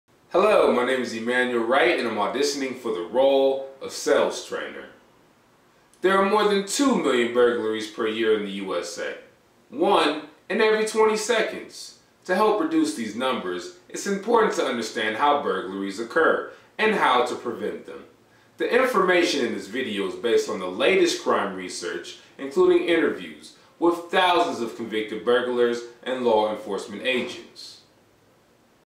Best Actor In The World Audition #3
Best Actor In The World Audition #3 When Your Still Using Home Phones In 2017 Der Witz Also known as Emmanuel Wright is a big fan of comedy and all things related. He is a young actor, and comedian on the rise living in Los Angeles California.